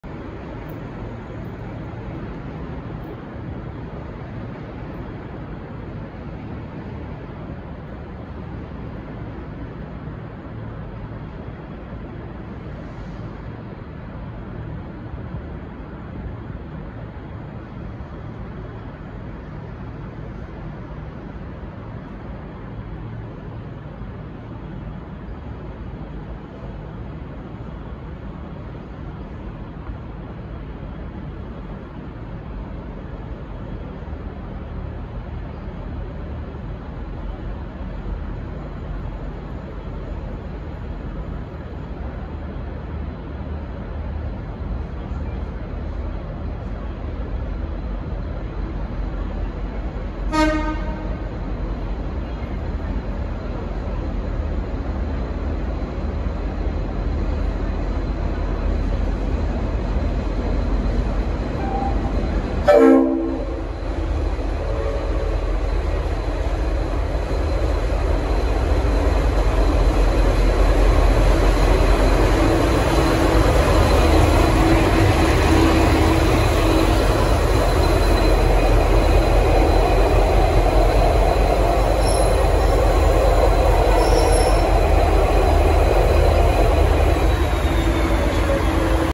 V/Line N452 Rural City of Wodonga Arriving at Southern Cross Station from Bacchus Marsh
V/Line N452 Rural City of Wodonga Arrives into Southern Cross Station from the Up Bacchus Marsh to Melbourne Service and I got a bit of a Leslie RS5T HORN SHOW from my Friendly Train Driver Thank you Mate👍👍👍👍👍👍👍👍👍👍😁😁😆😆😆😆😆😆😆😆😆😆😆😆😆😂😂😂😂😂😂😂😆😆😆😆😆😆😁😁😁😁😁😁😁😁😁😁😁👍👍👍👍👍👍👍👍👍👍👍👍

Train Loco: N452 Rural City of Echuca
Train No: #8222
Train Time: 8:08am
Train From: Bacchus Marsh
Date: May 24 2023
Location: Southern Cross Station